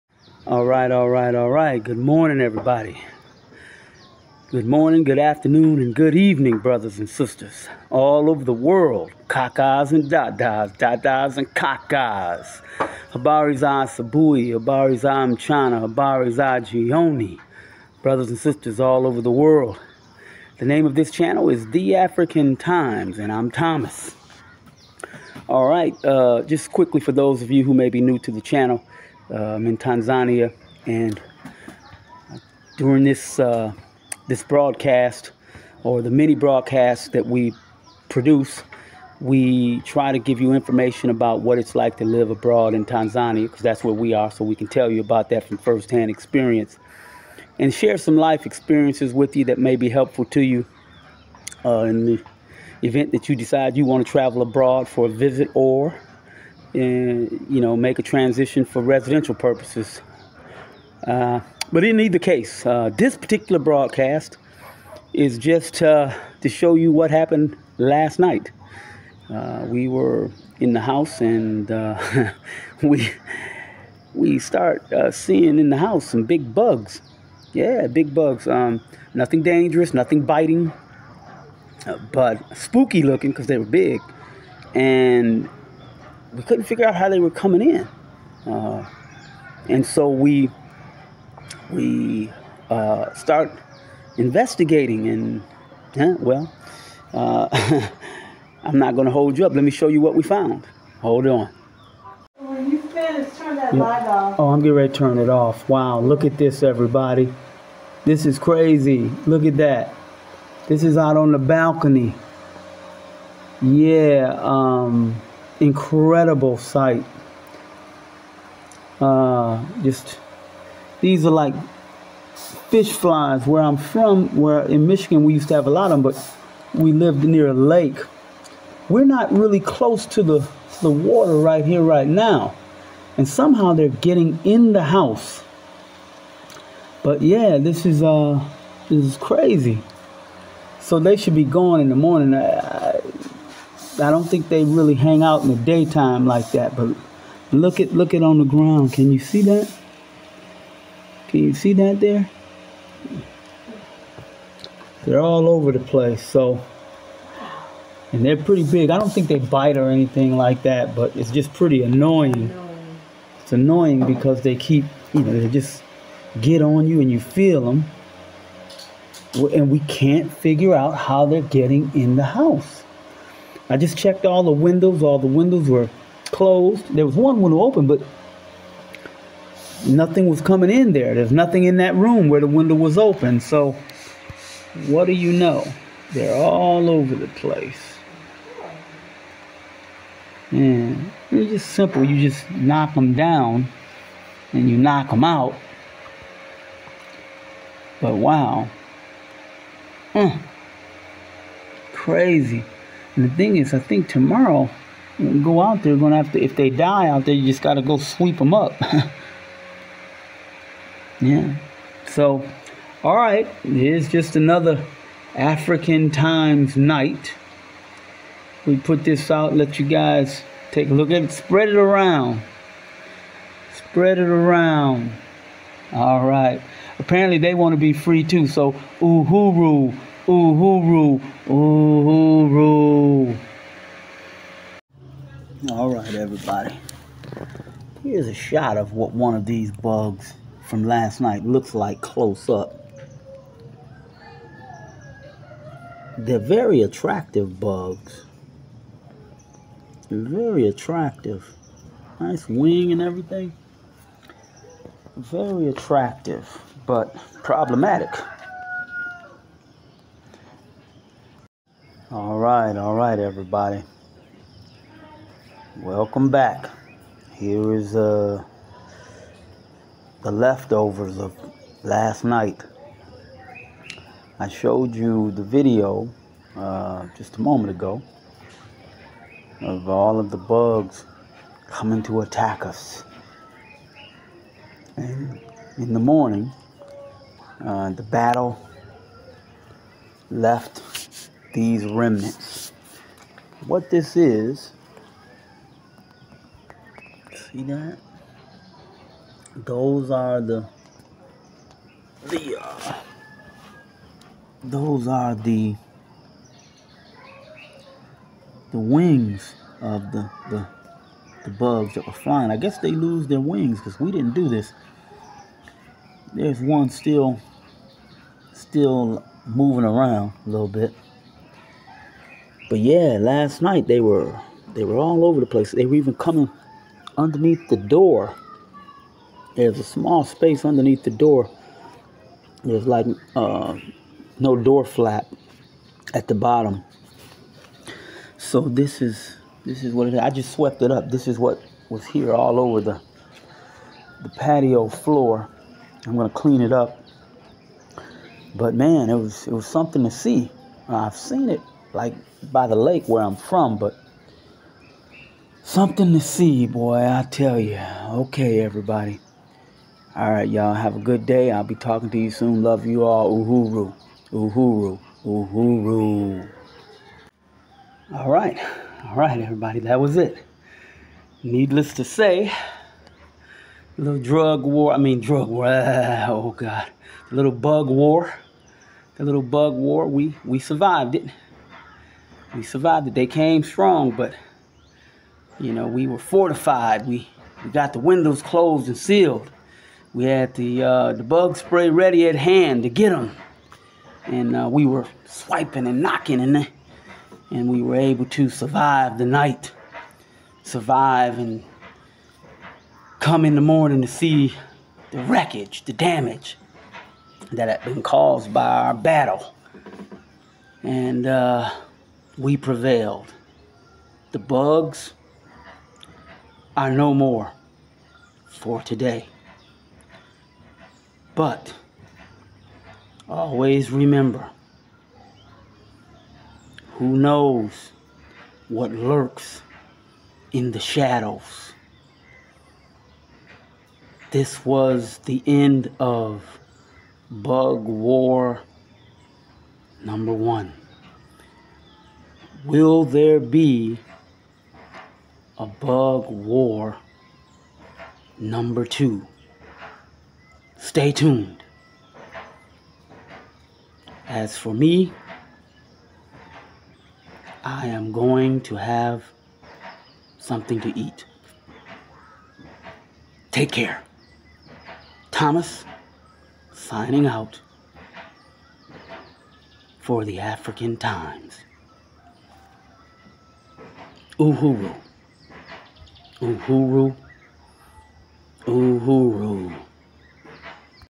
BREAKING NEWS:. AFRICA UNDER ATTACK!!?...THE BUG WAR HAS BEGUN...NOT REALLY
In this broadcast you see bugs invade my home.